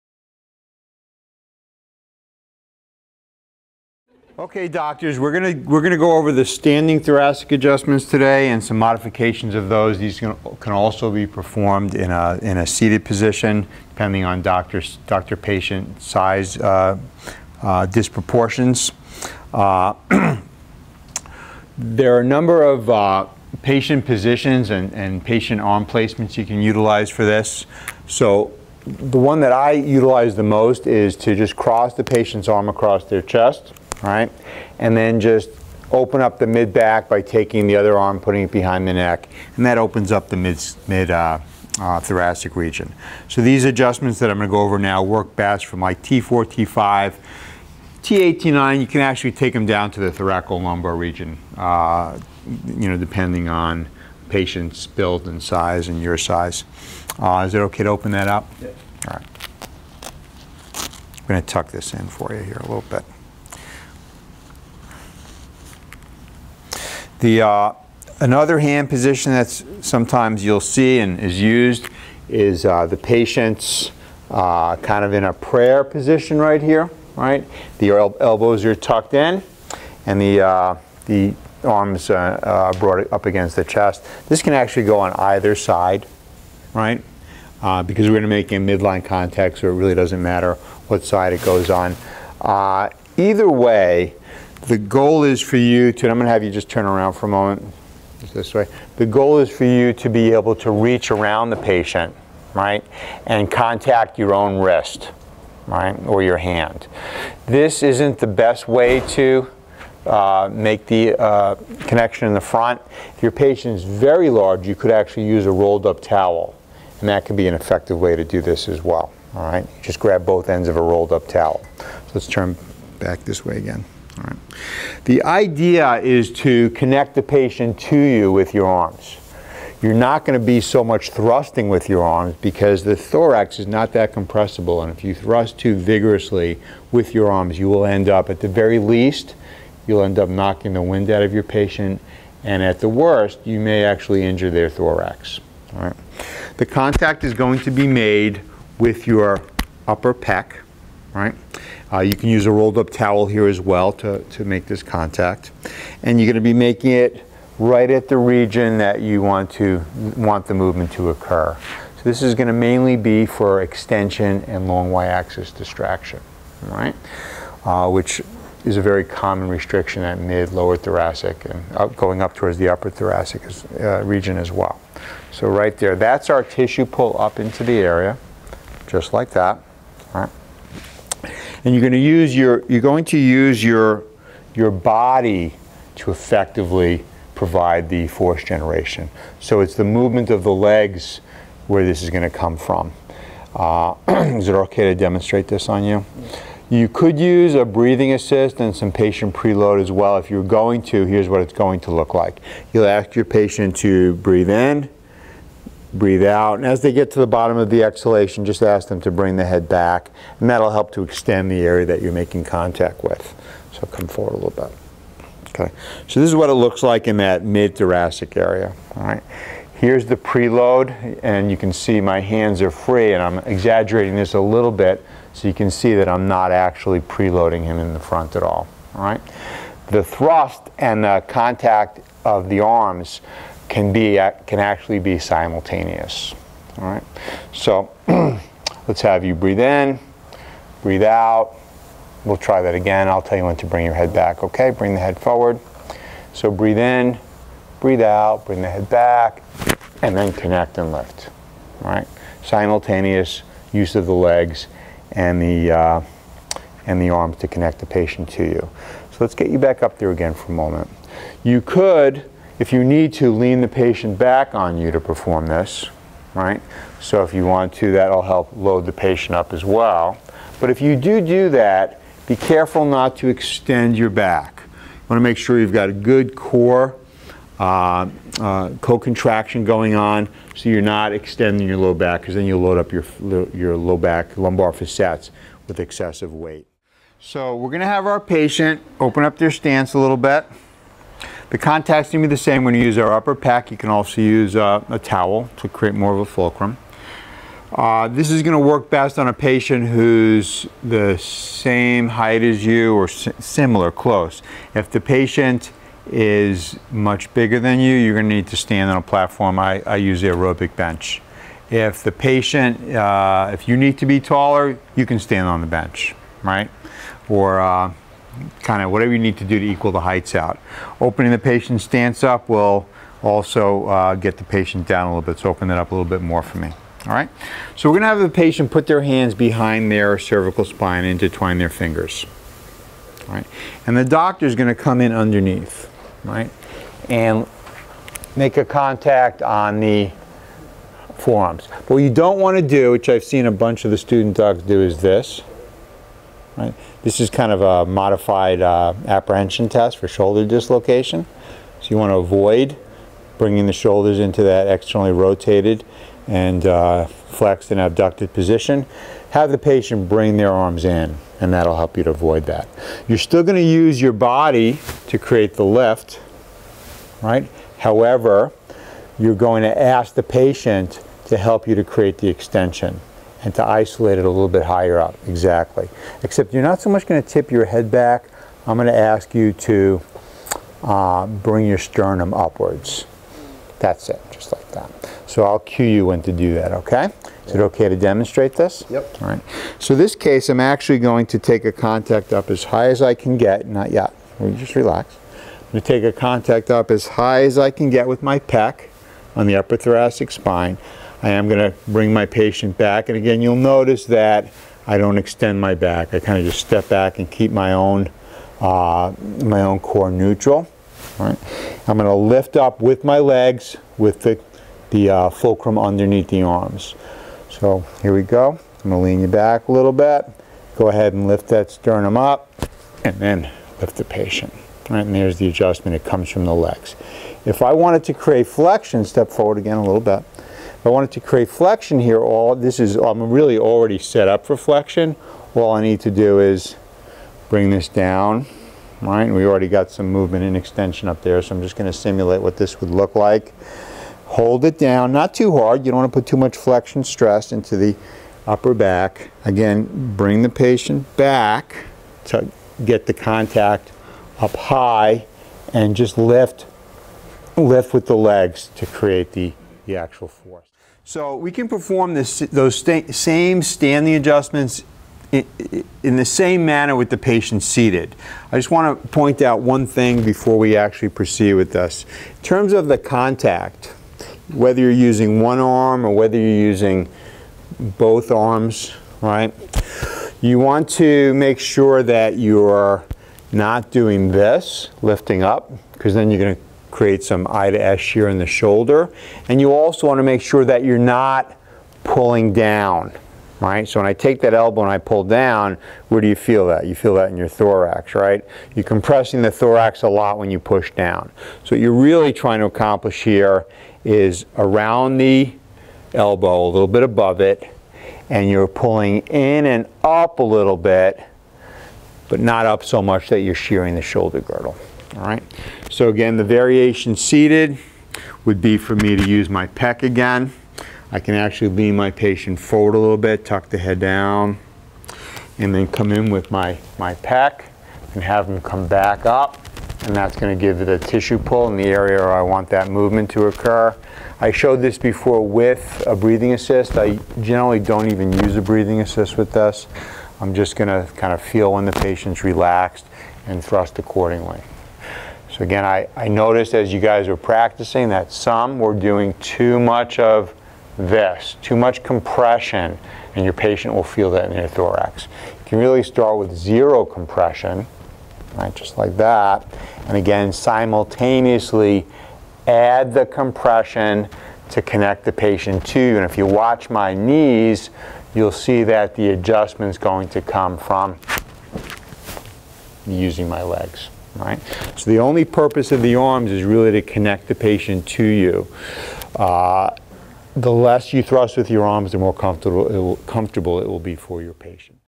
Mid Thoracic Manipulation 2
This is video 2 of 2 classes which provides instruction in the art of manipulating the thoracic spine.